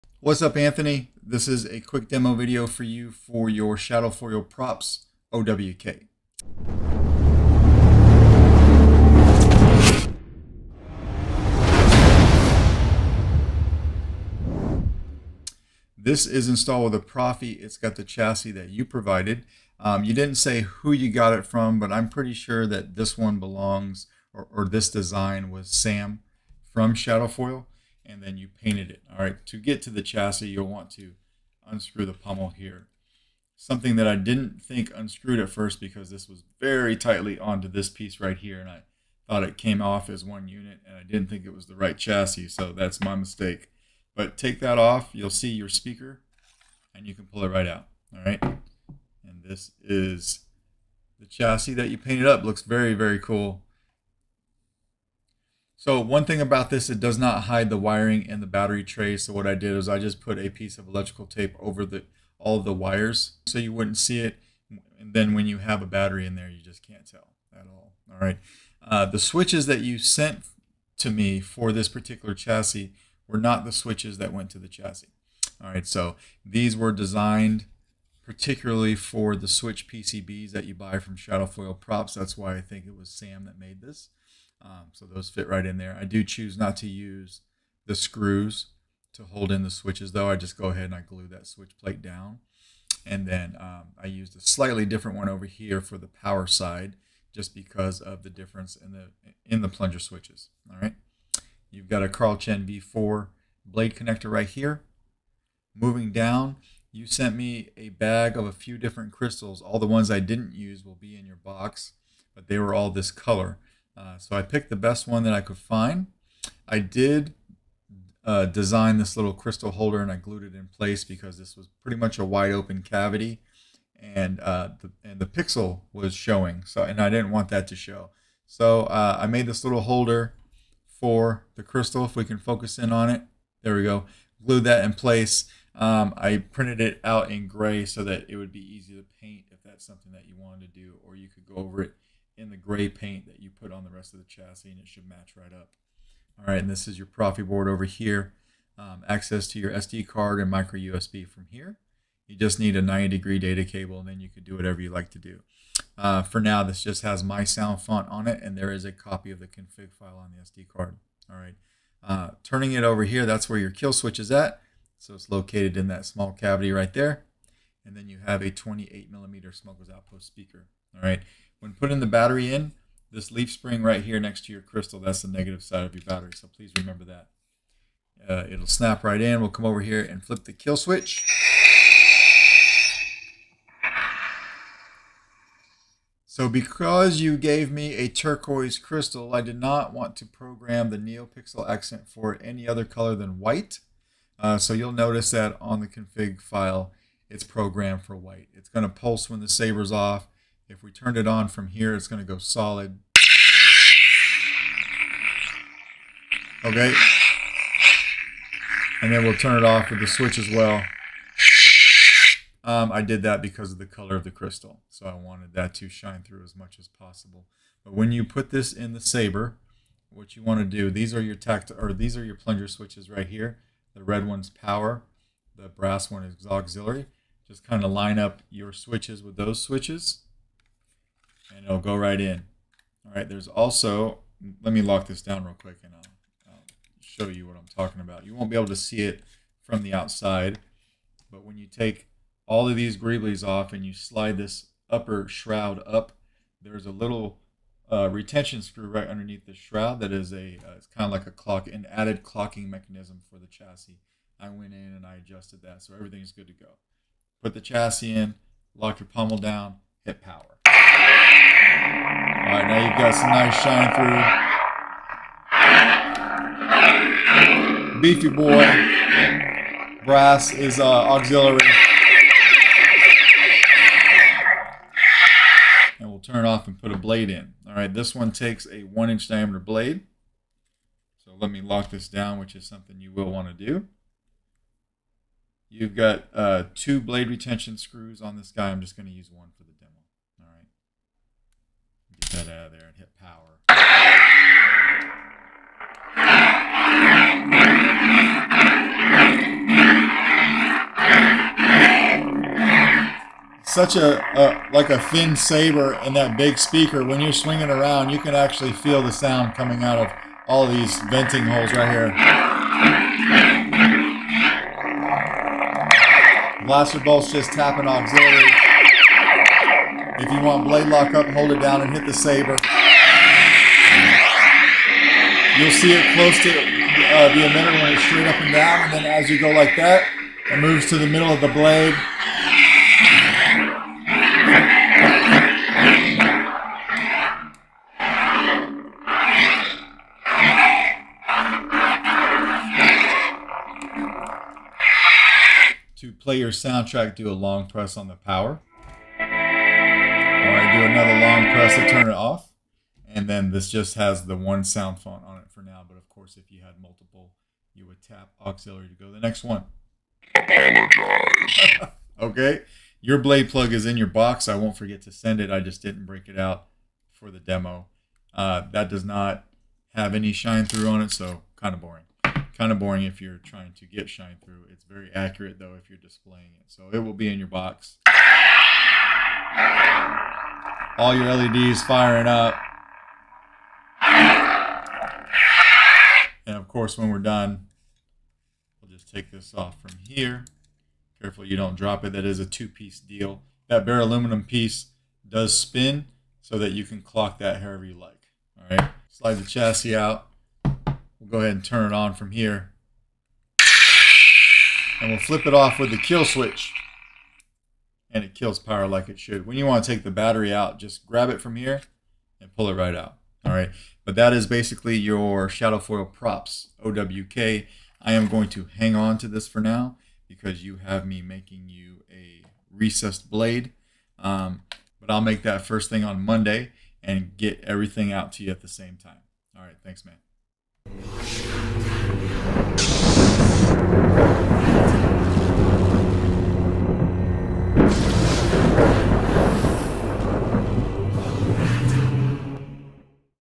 Anthony's Shadowfoil Props OWK Neopixel Lightsaber with Proffie
Installed with a Proffie and 28mm Smuggler's Outpost speaker with Shadowfoil Chassis